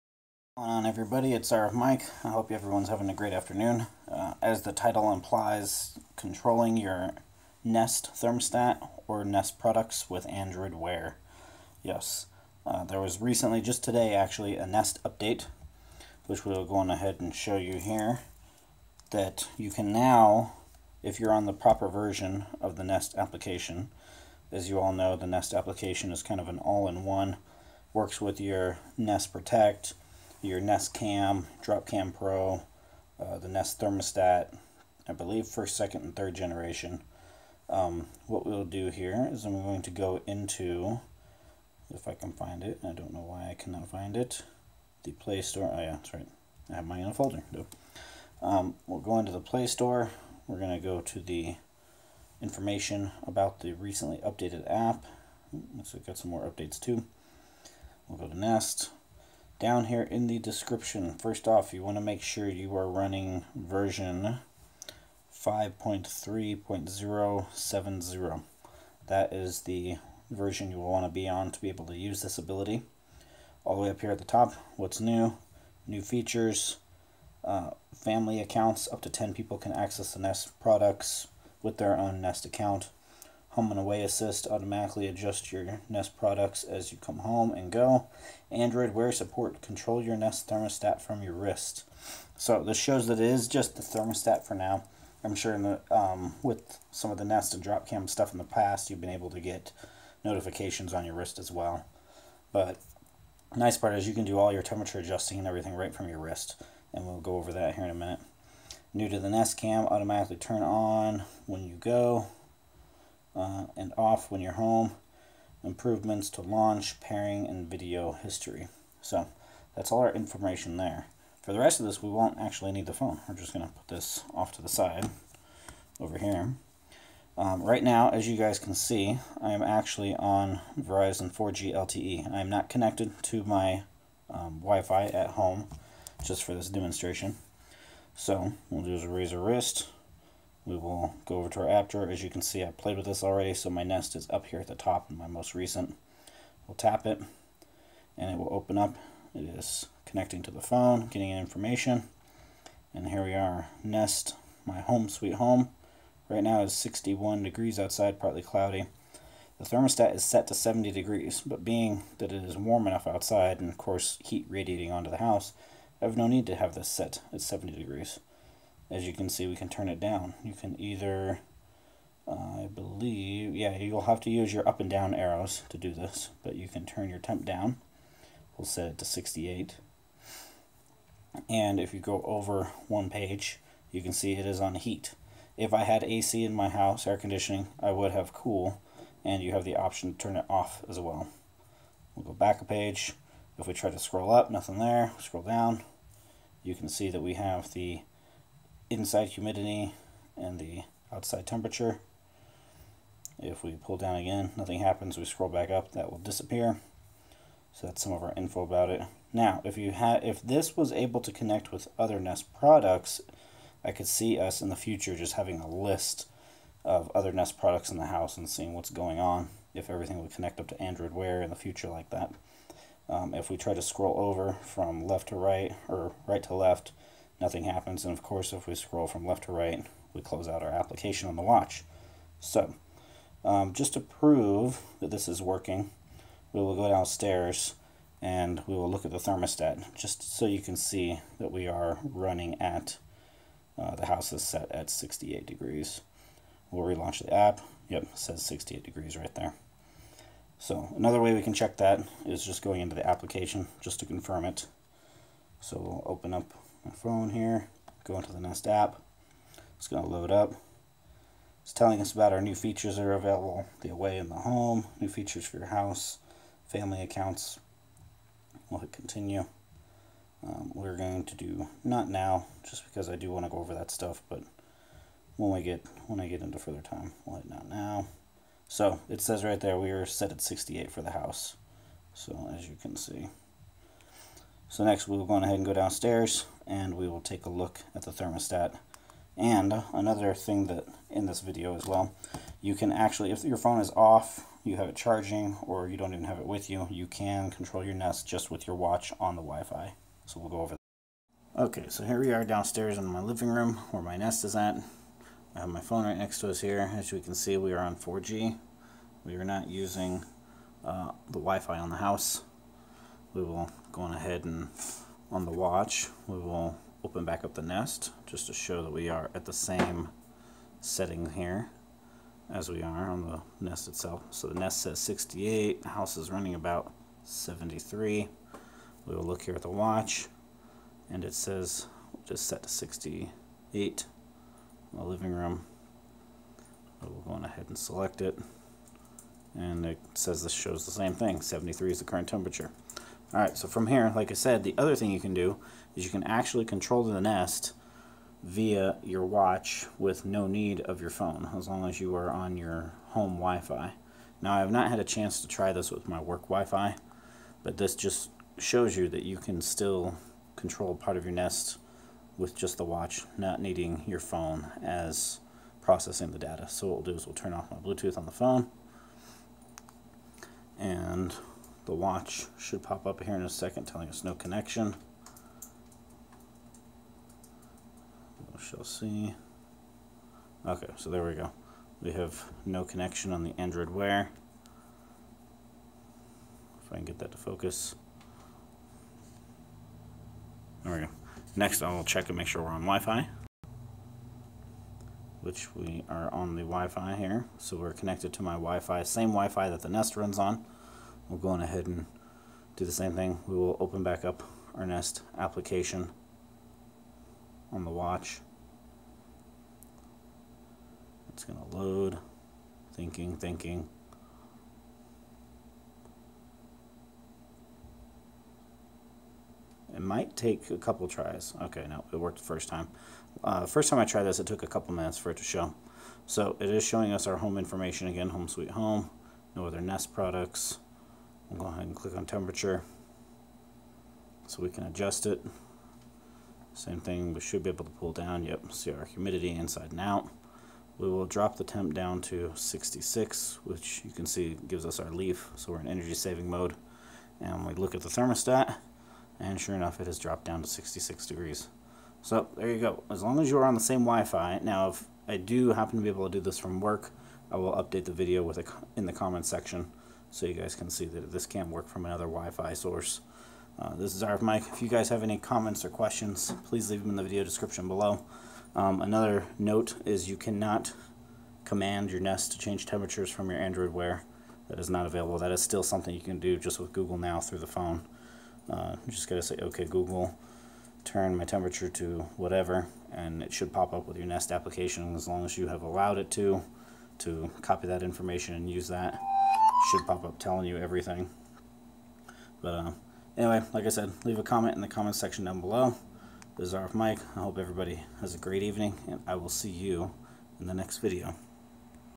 Controlling your Nest with Android Wear
YES YOU CAN. control your Nest that is...just with your watch...Or if your not at home you can still control it through your watch Via your phone.
 IF you can control your nest via a work wifi i will update "THIS AREA" of the Comments section.